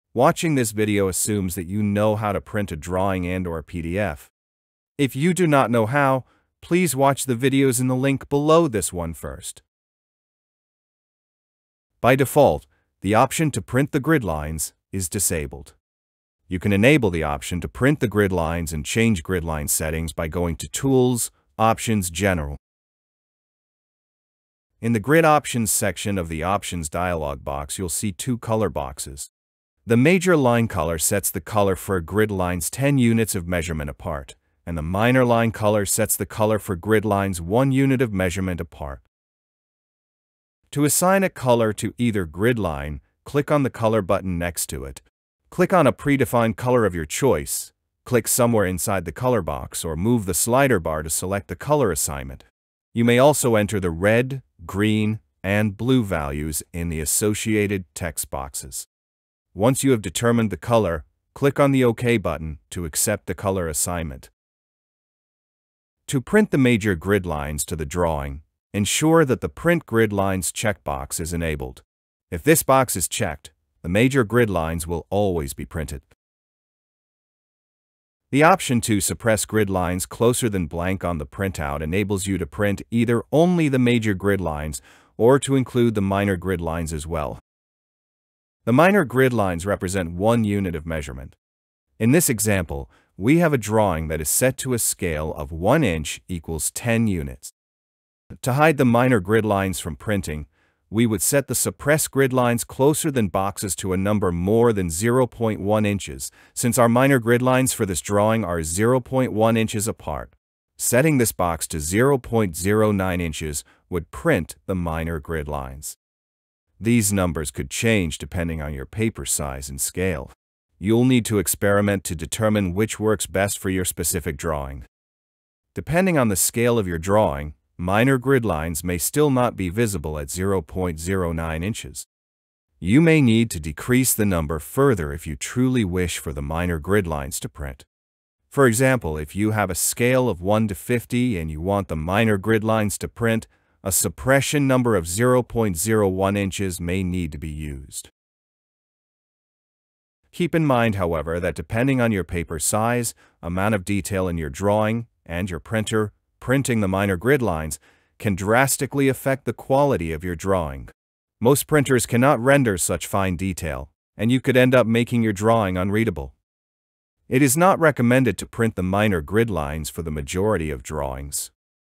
Printing the Grid Lines
Learn how to print the major and minor grid lines onto your drawing.